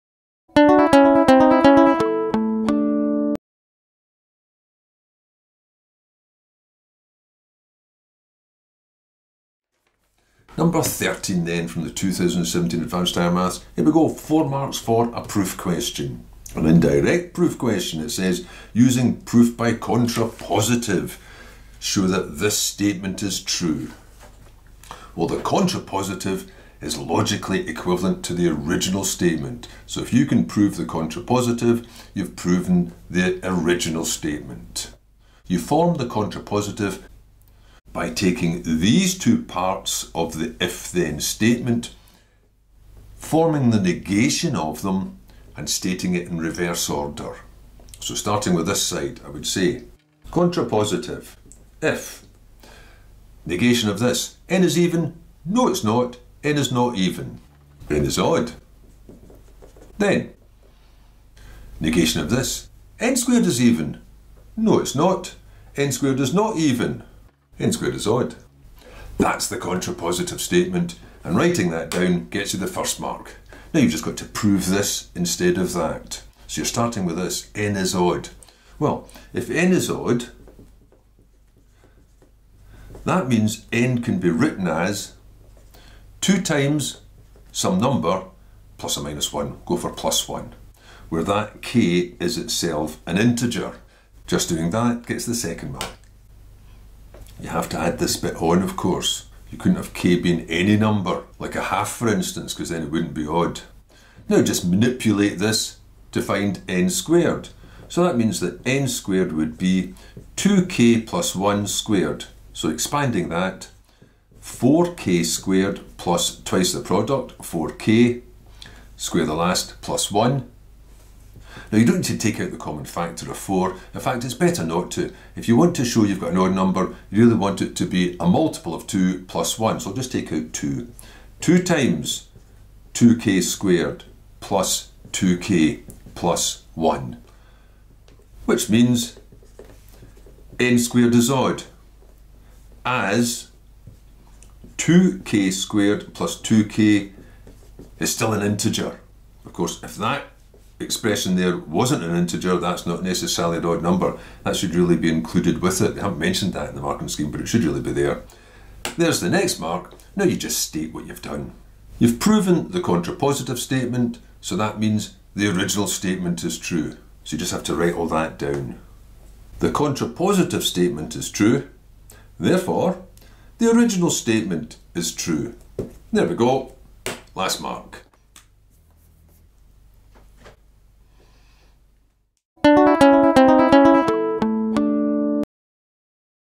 2017 SQA Advanced Higher Mathematics No. 13 Proof by contrapositive
Prove a given statement using proof by contrapositive.

Instructional exercise consisting of question 13 from the 2017 SQA Advanced Higher Mathematics examination.